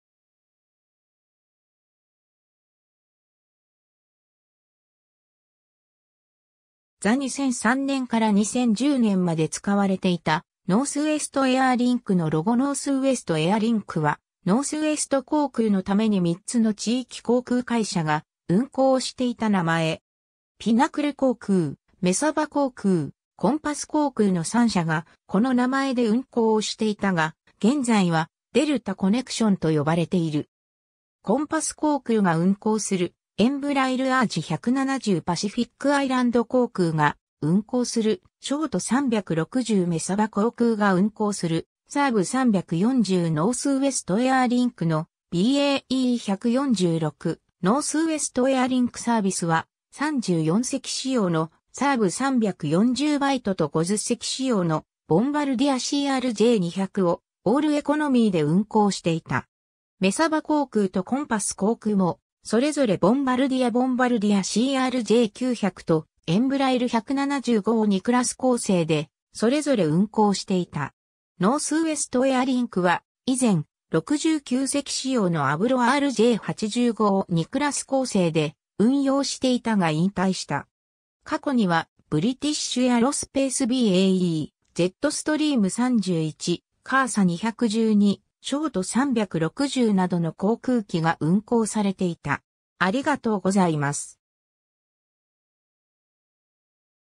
ノースウエスト・エアーリンク
The 2003年-2010年まで使われていたノースウエストエアーリンクのロゴ ノースウエスト・エアリンク(Northwest Airlink)はノースウエスト航空のために3つの地域航空会社が運航をしていた名前。
ピナクル航空、メサバ航空、コンパス航空の3社が、この名前で運航をしていたが、現在はデルタ・コネクションと呼ばれている。
 コンパス航空が運行するエンブラエルERJ170 パシフィックアイランド航空が運行するショート360 メサバ航空が運行するサーブ340 ノースウエストエアーリンクのBAe 146 ノースウエストエアリンクサービスは、34席仕様のサーブ 340Bと50席仕様のボンバルディア CRJ200をオールエコノミーで運行していた。
メサバ航空とコンパス航空も、それぞれボンバルディアボンバルディア CRJ900とエンブラエル 175を2クラス構成でそれぞれ運行していた。
 ノースウエストエアリンクは以前、69席仕様のアブロ RJ85を2クラス構成で運用していたが引退した。
過去には、ブリティッシュ・エアロスペースBAe ジェットストリーム 31、CASA 212、ショート 360などの航空機が運行されていた。